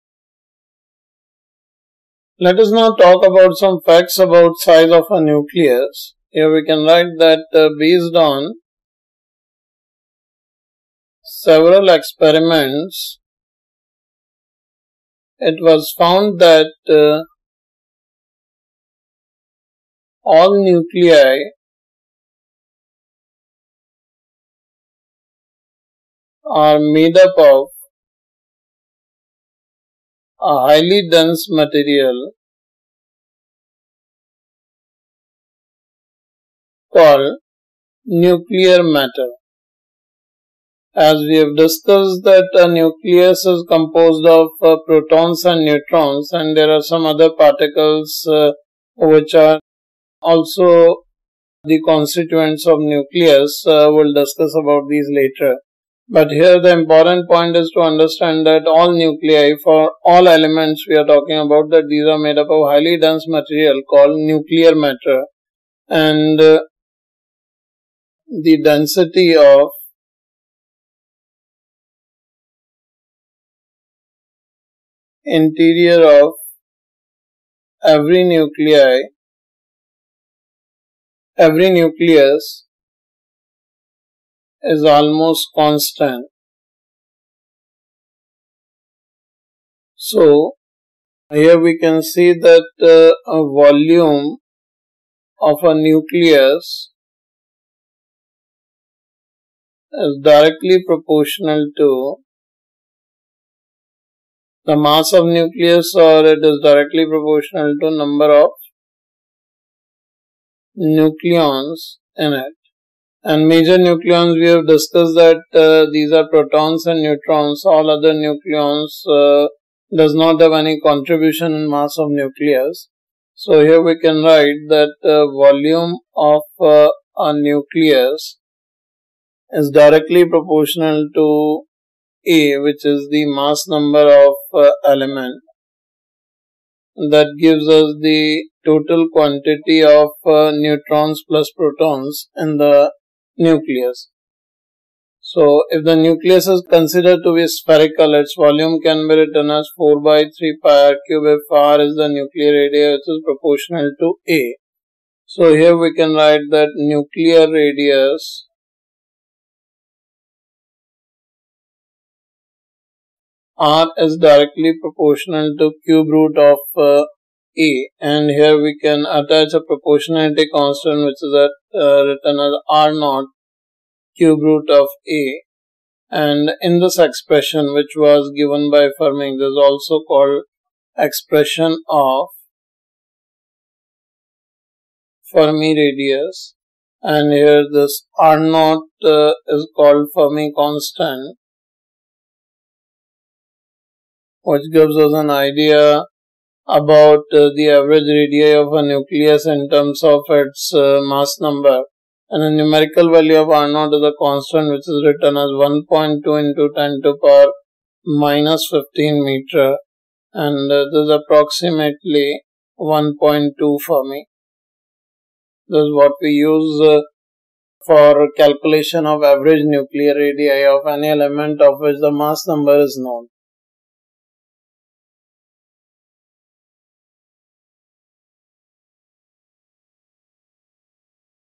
Class 12 Physics | Nuclear Stability | #3 Size of a Nucleus | For JEE & NEET
PG Concept Video | Radioactivity | Size of a Nucleus by Ashish Arora

Students can watch all concept videos of class 12 Radioactivity for jee & neet in proper sequence through PG channel playlist.